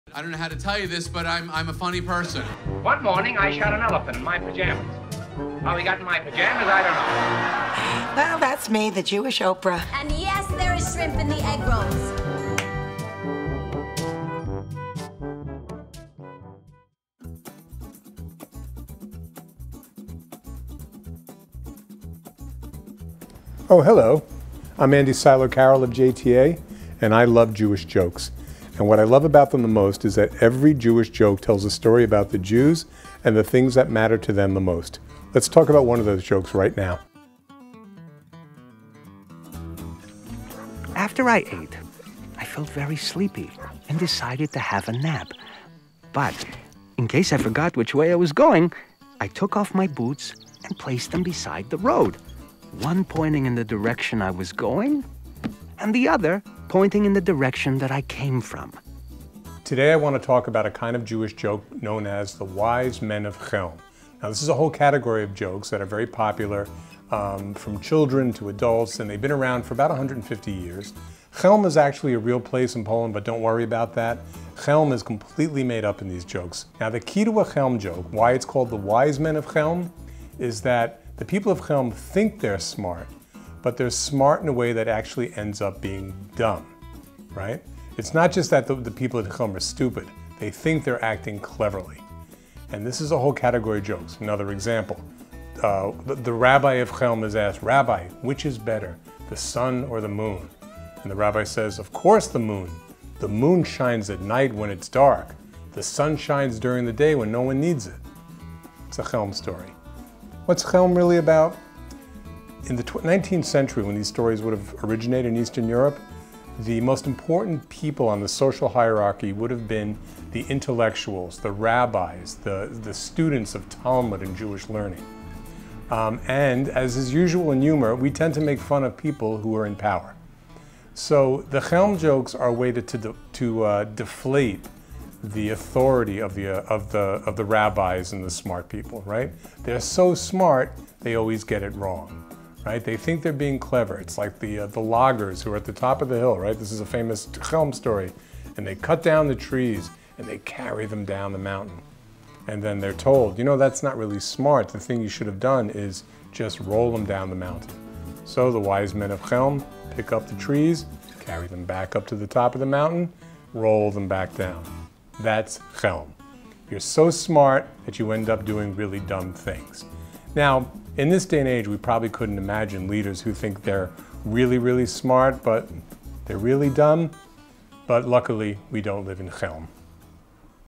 "Chelm Jokes"
Shtick: Jewish Jokes Explained -- JTA's editor-in-chief Andrew Silow-Carroll walks you through a classic Jewish joke and explains what it's all about.